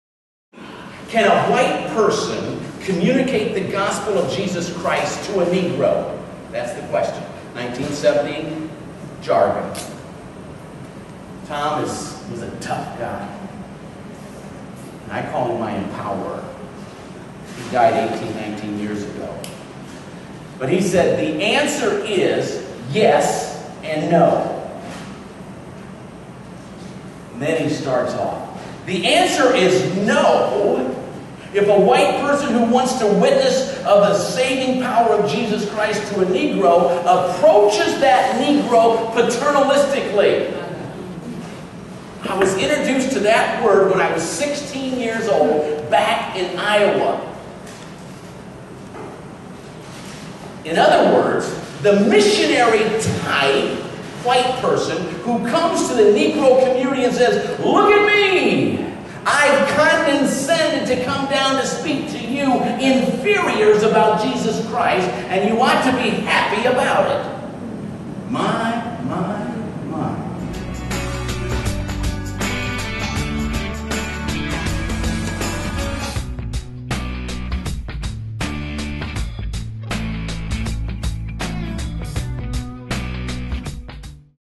Can Whites Witness to Blacks?
Pastor Wayne Gordon was host for the 2012 CCDA Immersion group in Chicago.  He challenged the group with a question;  Can White people share Jesus with Black people?  This video is posted by DoYouKnowMe.org to stimulate healthy conversation and challenge thinking.